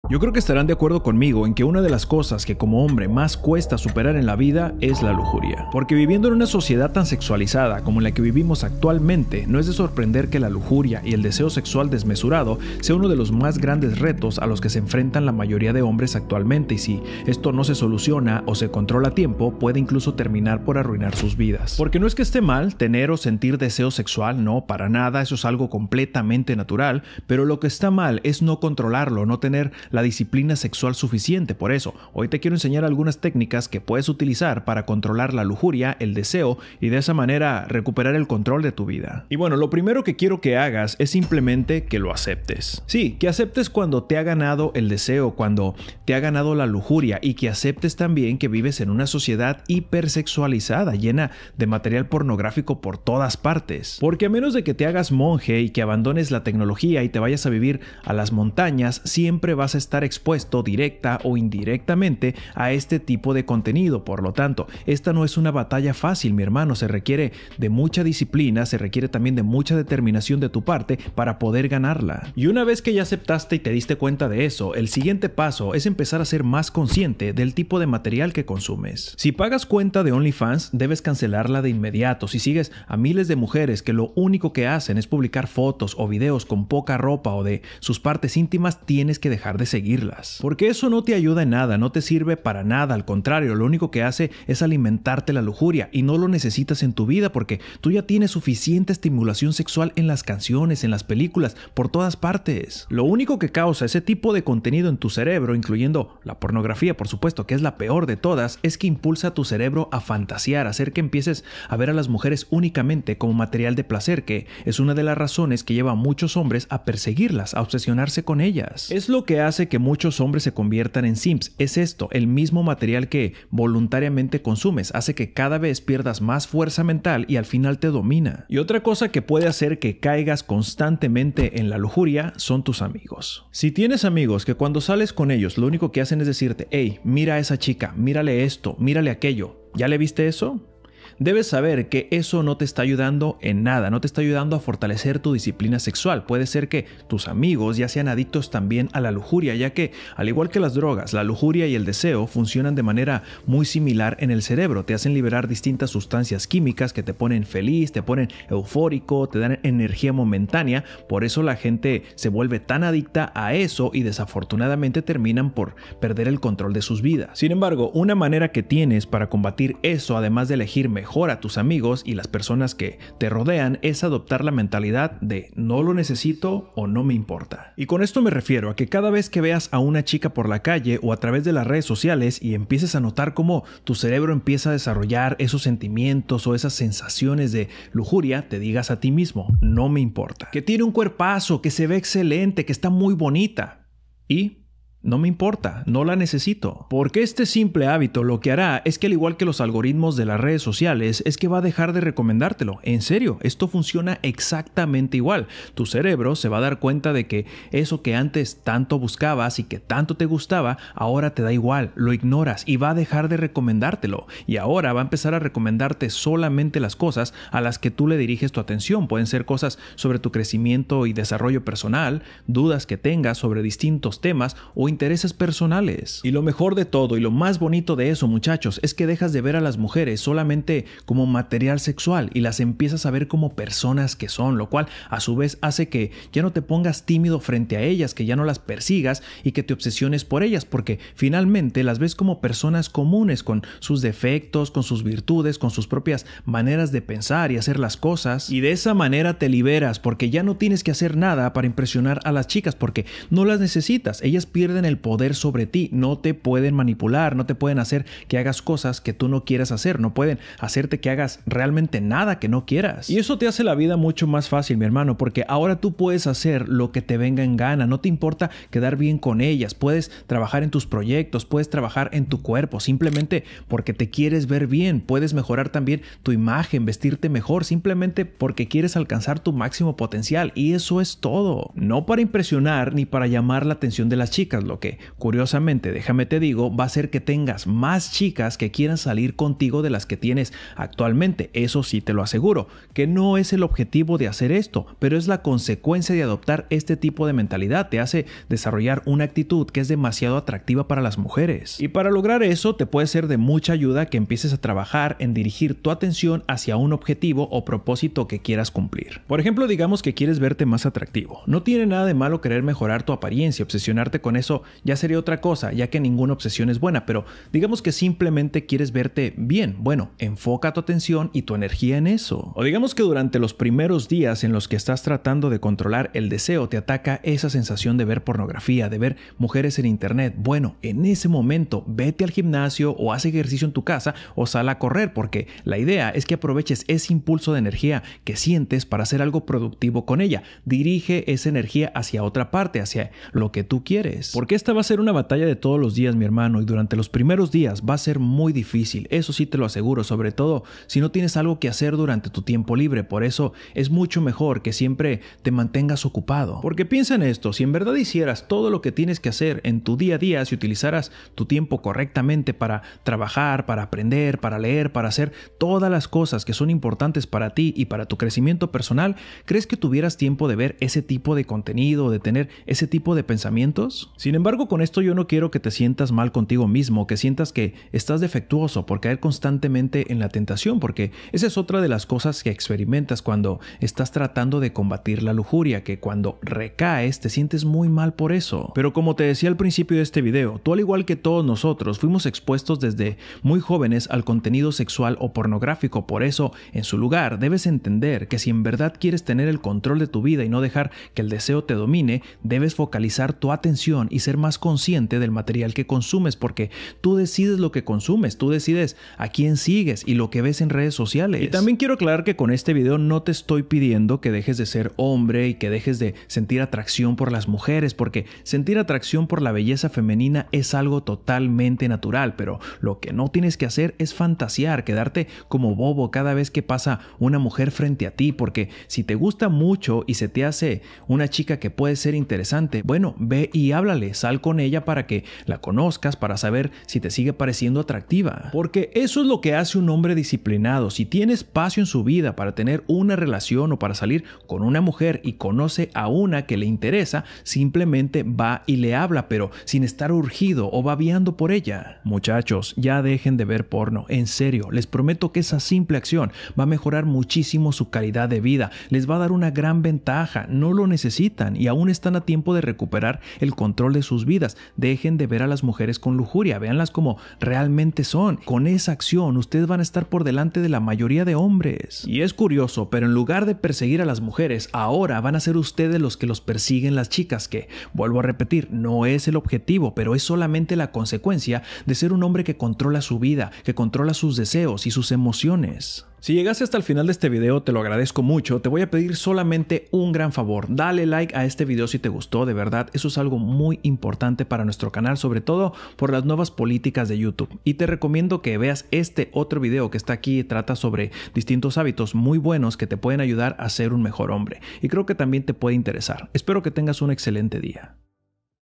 Esta es la DEBILIDAD Más Grande De Un Hombre ¡Supérala Ya!
7 HÁBITOS que te hacen DÉBIL
Este es uno de los más grandes retos a los que se enfrenta la mayoría de hombres actualmente, y si no se controla puede arruinar sus vidas. Por eso, hoy te quiero enseñar algunas técnicas que puedes utilizar, para controlarlo y de esa manera recuperar el control de tu vida.

También puedes apoyar nuestro canal presionando el botón de Súper Gracias ❤️ 🙏
Abrazos y mucha buena vibra.
- Misaglez

                 🔺  VISITA MI TIENDA! 👇